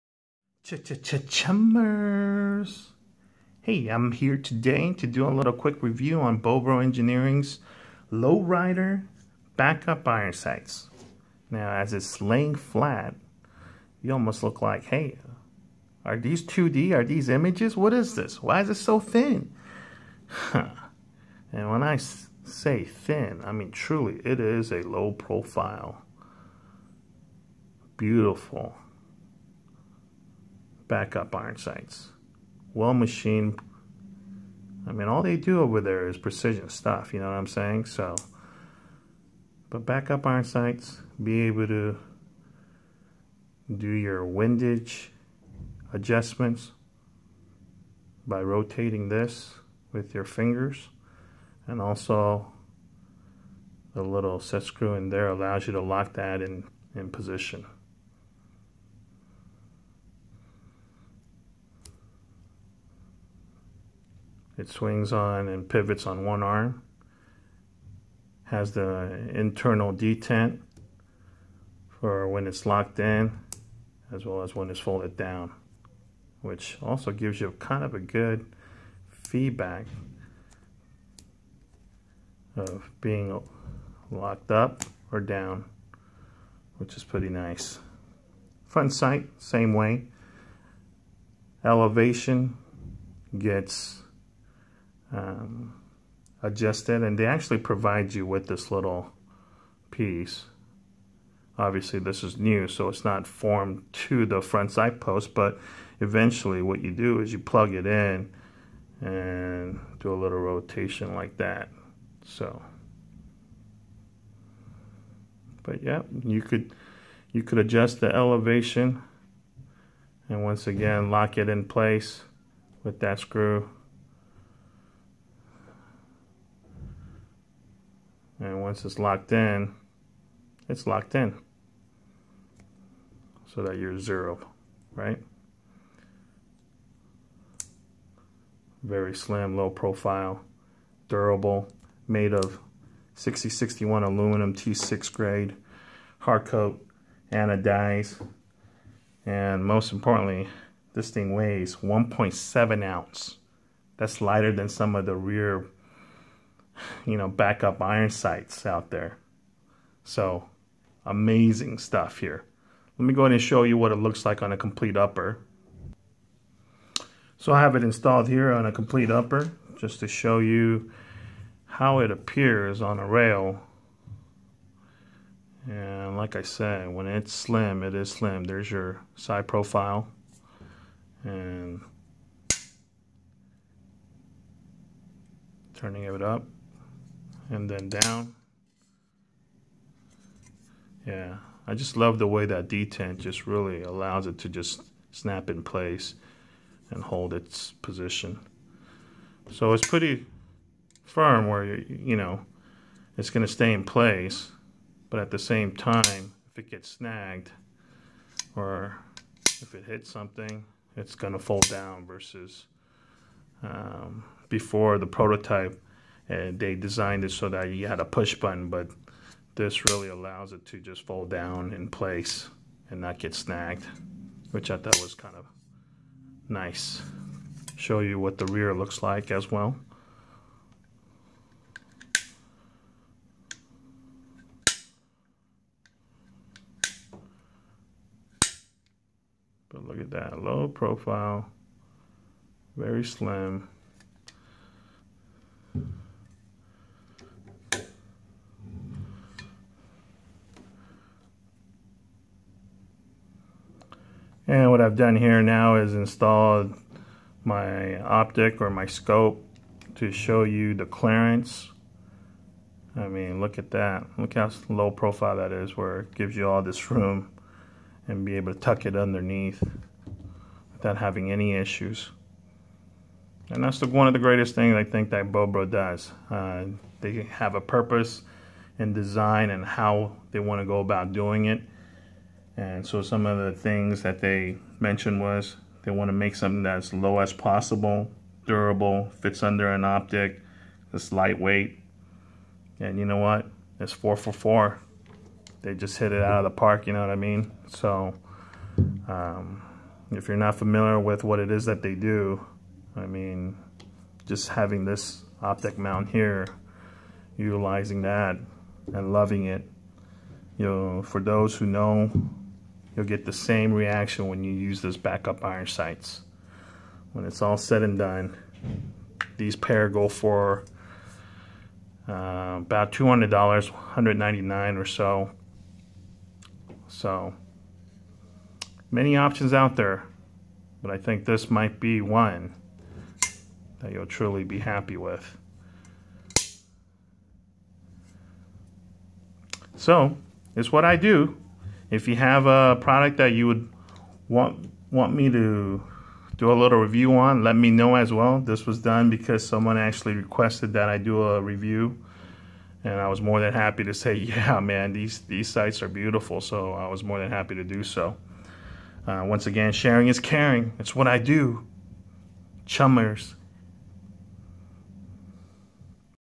Bobro Engineering LowRiders BUIS
Chummerz review on Bobro Engineering Lowrider Back Up Iron Sights. These are the slimmest and lightweight BUIS out there. Durable and well machined sights weigh in at 1.7oz made from 6061-T6 Aluminum and anodized to Mil Type III Class 2. These run around $200 for the set and IMO well worth every penny.